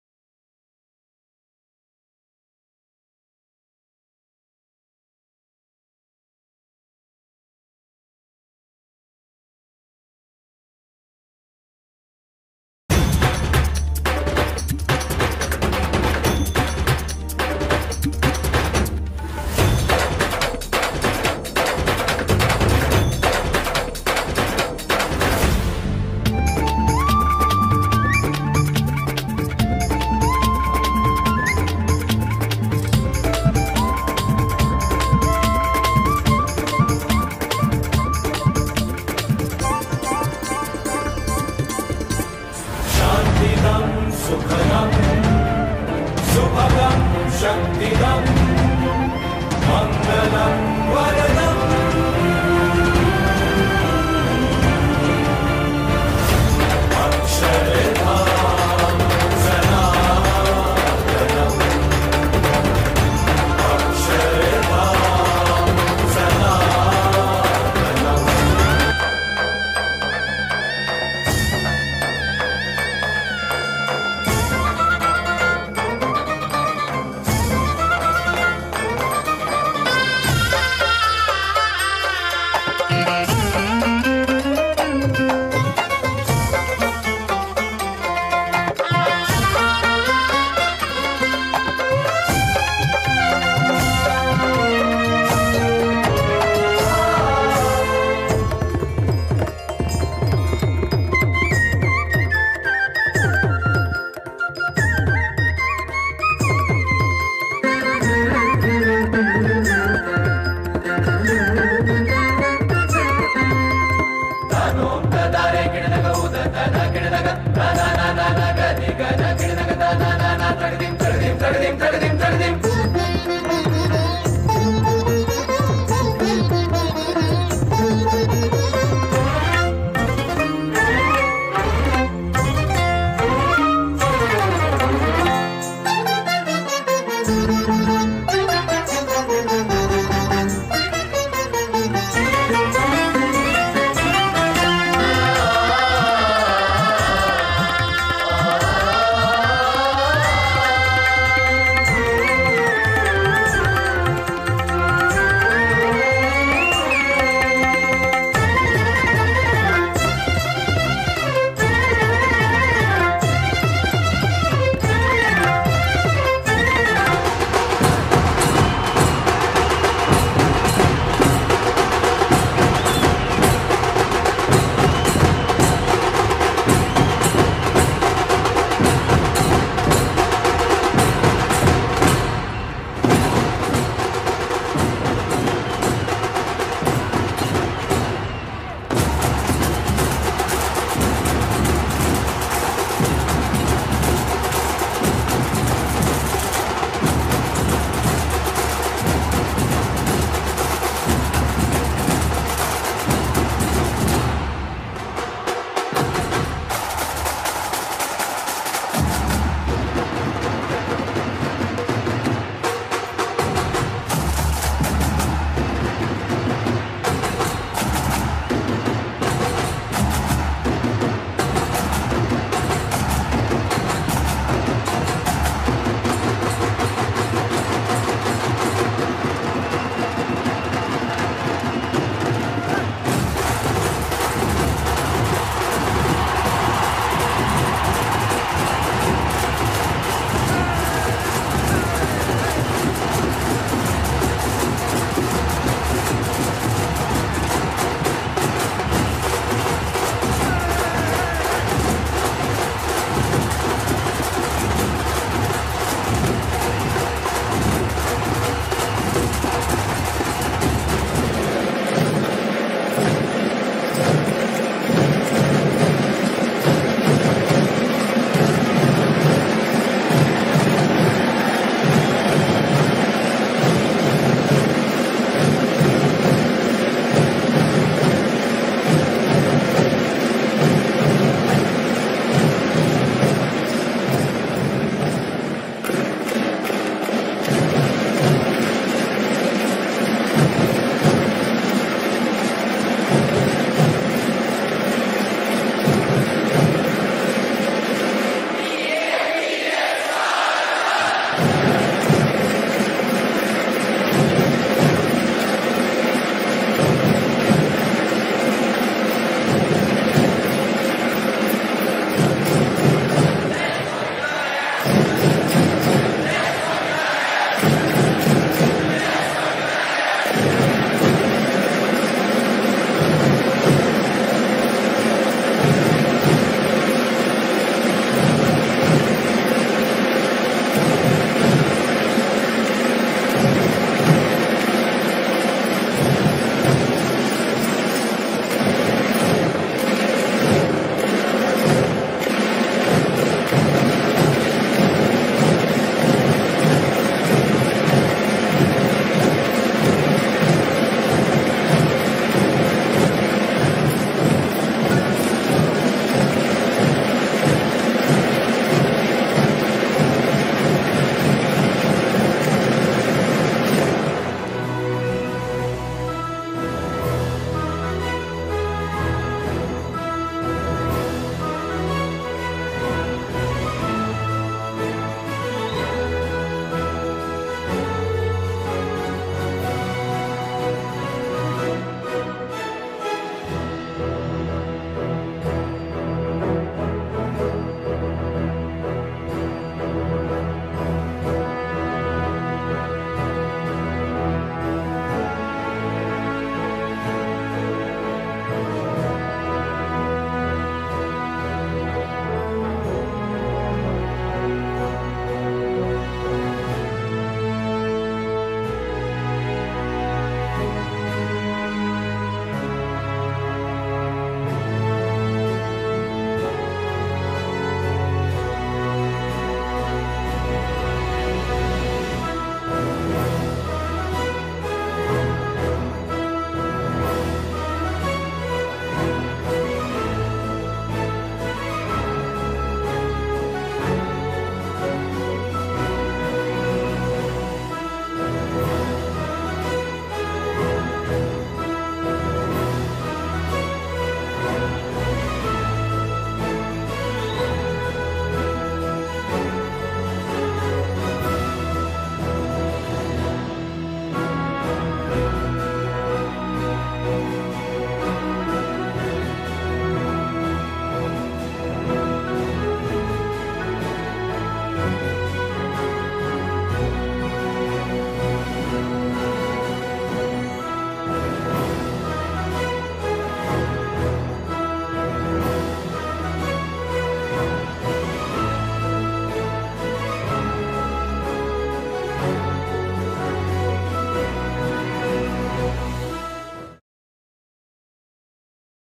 Light & Sound Show Promo, BAPS Gharmandir, Bahrain
BAPS Gharmandir Light & Sound Show Promo. First light and sound show will be in the month of June. To watch all the videos of light and sound show go to this link given below.
list=PL3zvQzJUBNPjj3kghinXONxKilQfxp4K1